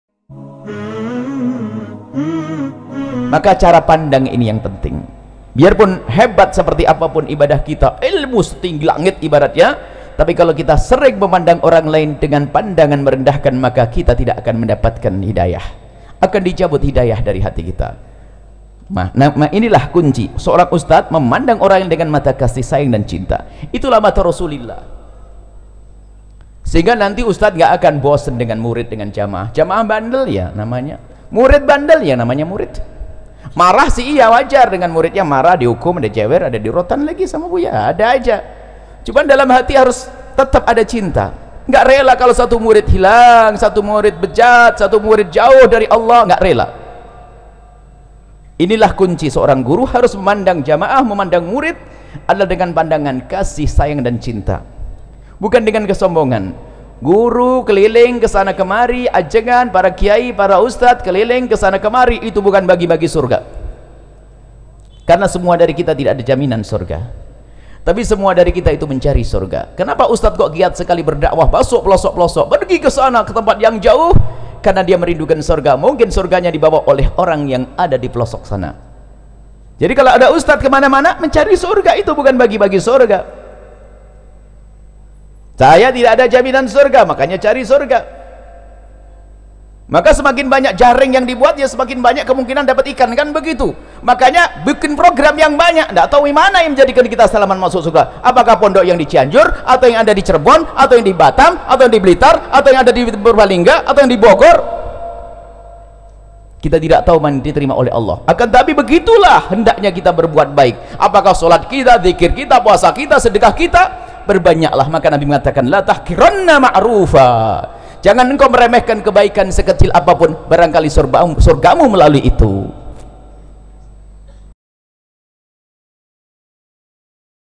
Alasan Ustad Berdakwah - Hikmah Buya Yahya
Majelis Al-Bahjah Cianjur Bersama Buya Yahya | 26 Syawal 1439 H / 10 Juli 2018

INFORMASI INFAQ CENTRE AL-BAHJAH :

No. Rek : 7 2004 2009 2
KODE BANK : 451
a/n : Yayasan Al Bahjah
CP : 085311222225